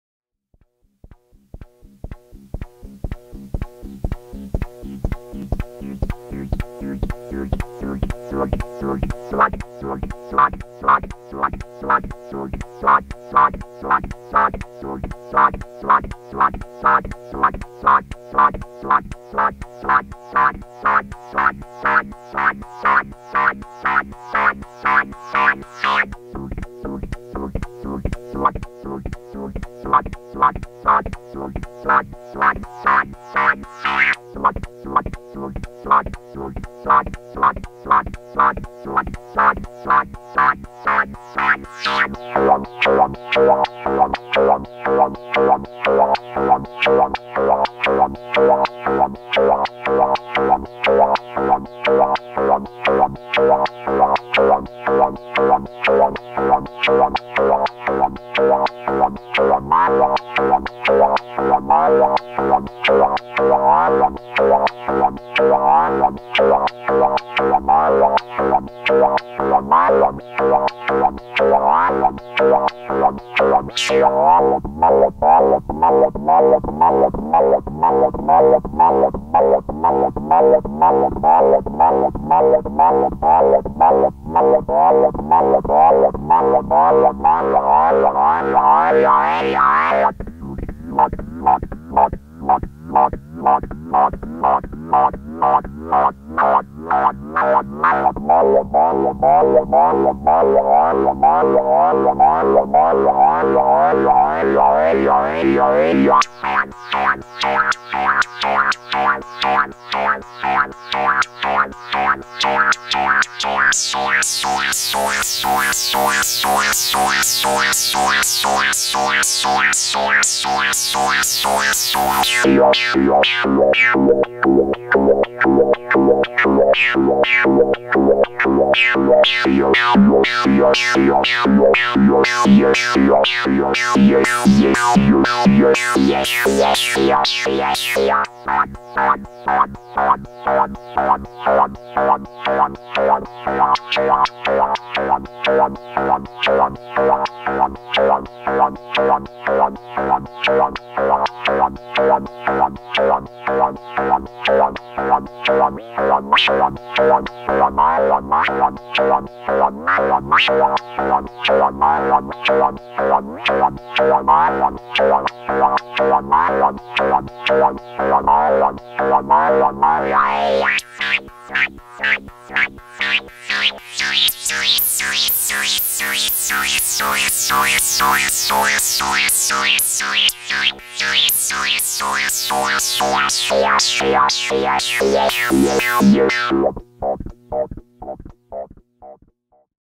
Korg MS-20 snappy loop
it's a kind of envelope modulated loop with ESP and EG1.
pitch bend is controlled by EG1 knobs.
snappy filter resonance sounds are one of the characteristic tones of MS-20.